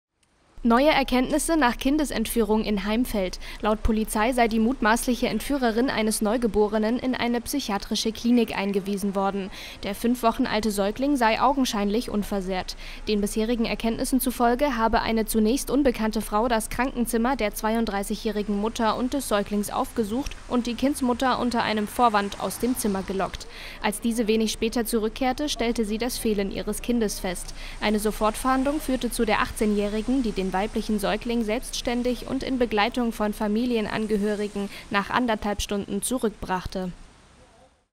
Nach Kindesentführung: 18-Jährige in psychiatrischer Klinik
Neue Erkenntnisse nach Kindesentführung in Heimfeld: Laut Polizei sei die mutmaßliche Entführerin eines Neugeborenen in eine psychiatrische Klinik eingewiesen worden. Der 5-Wochen-alte Säugling sei augenscheinlich unversehrt. Den bisherigen Erkenntnissen zufolge habe eine zunächst unbekannte Frau das Krankenzimmer der 32-jährigen Mutter und des Säuglings aufgesucht und die Kindsmutter unter einem Vorwand aus dem Zimmer gelockt. Als diese wenig später zurückkehrte, stellte sie das Fehlen ihres Kindes fest. Eine Sofortfahndung führte zu der 18-jährigen, die den weiblichen Säugling selbstständig und in Begleitung von Familienangehörigen nach anderthalb Stunden zurück brachte.